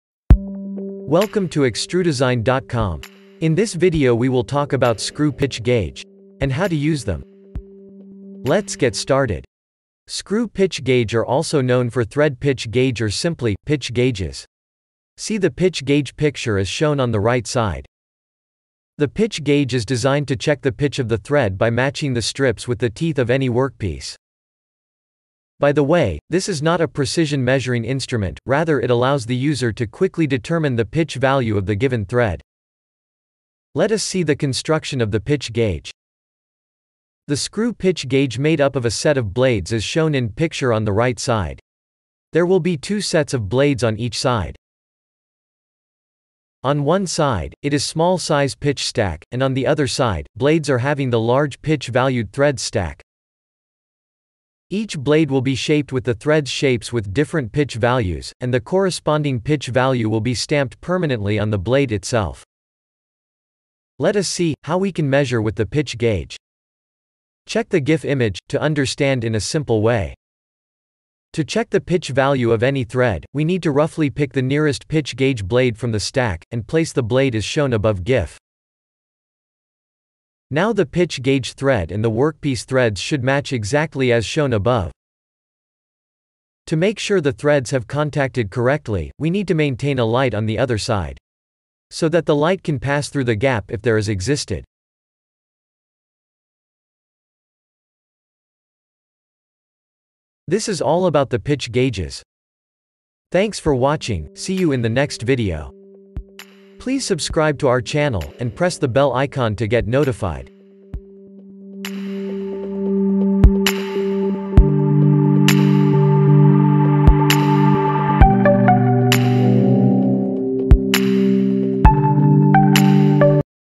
How to use Screw Pitch gauge? | Thread Pitch Gauge
The screw pitch gauge is also known for thread pitch gauge. Simply pitch gauge.

In metrology, Screw pitch gauge/ Thread pitch gauge is designed to check the pitch of the thread by matching the strips with the teeth of any workpiece.

This is not a precision measuring instrument, rather it allows the user to quickly determine the pitch value of the given thread.